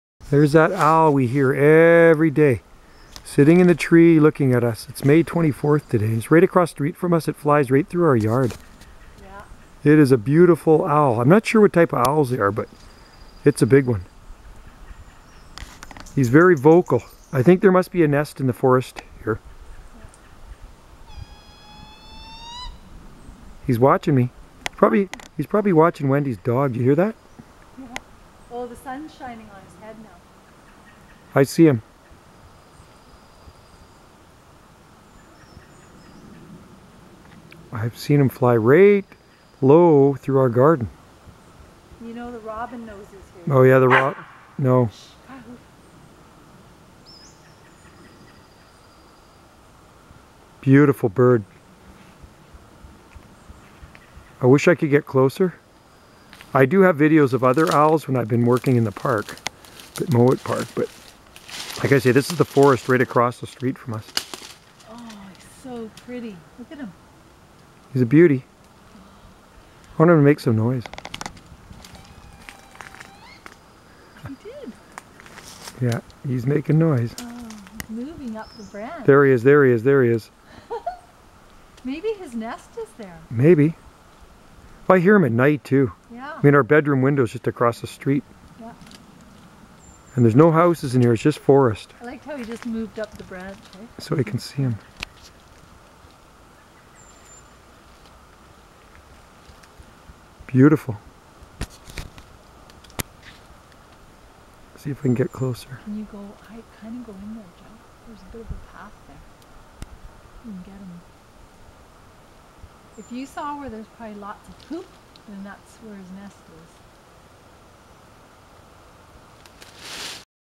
Barred Owl just outside our door. Resident Owl!
Loving Natures pleasant gifts. We have a resident Barred Owl that lives here with us on Salt Spring Island. Magnolia Petal Accommodation.